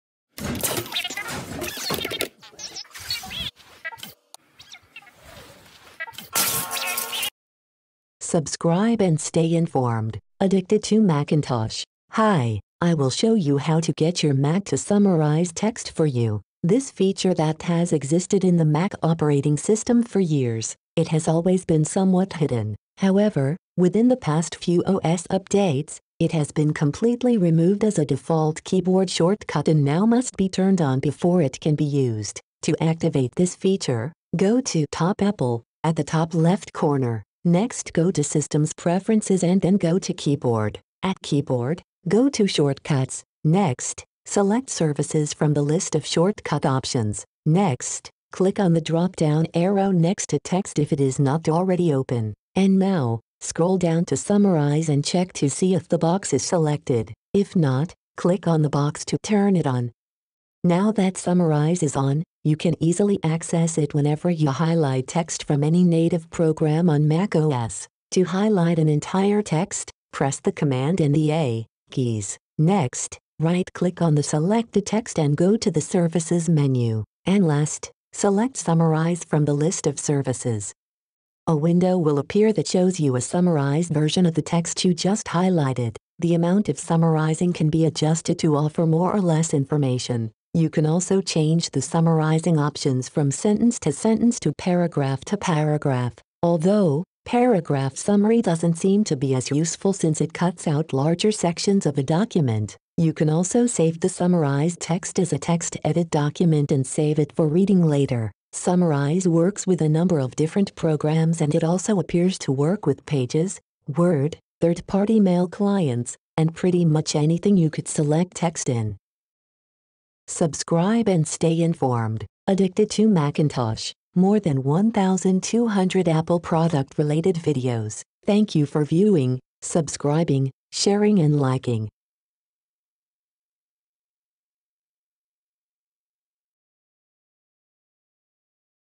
How to get your Mac to summarize text for you
You may get your Mac to summarize text for you, here is how it's done...